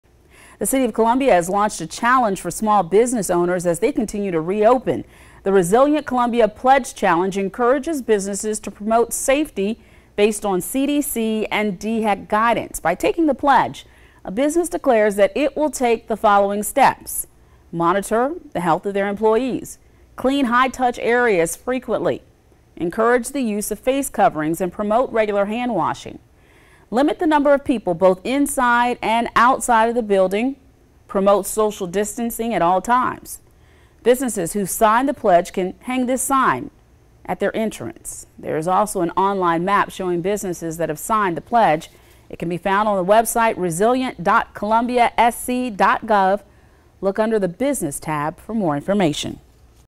City of Columbia launches 'Resilient Columbia Pledge Challenge' for small business owners
The challenge encourages reopening businesses to take steps based on CDC and DHEC guidance in light of the COVID-19 health crisis. More